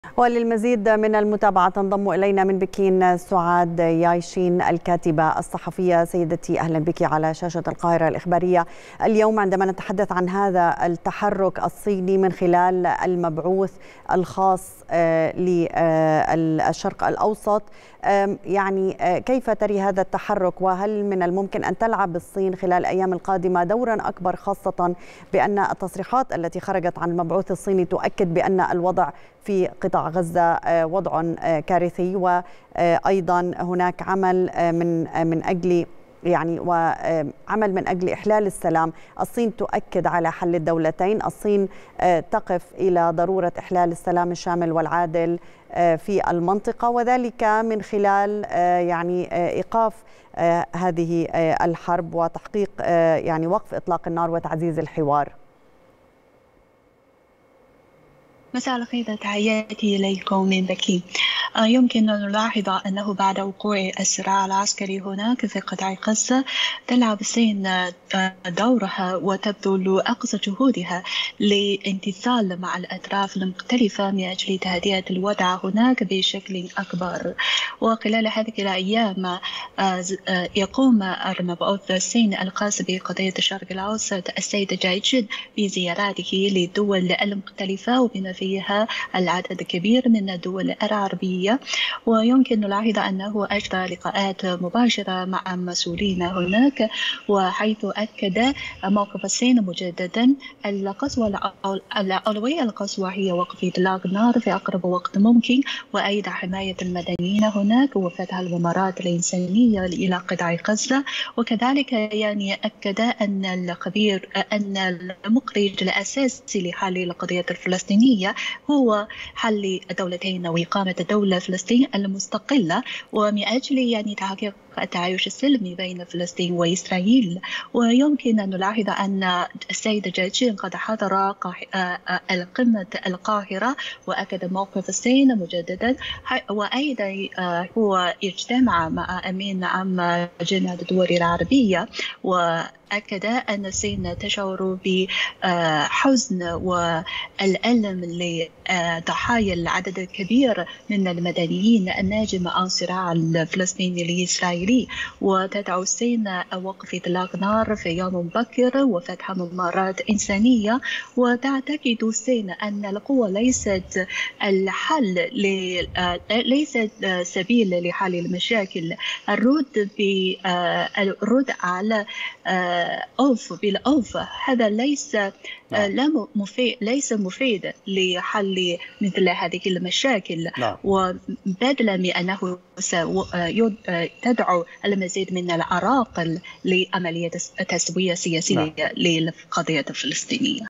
هل تلعب الصين دورا لوقف إطلاق النار في غزة؟.. سعاد ياي شين الكاتبة الصحفية تٌجيب
23/10/2023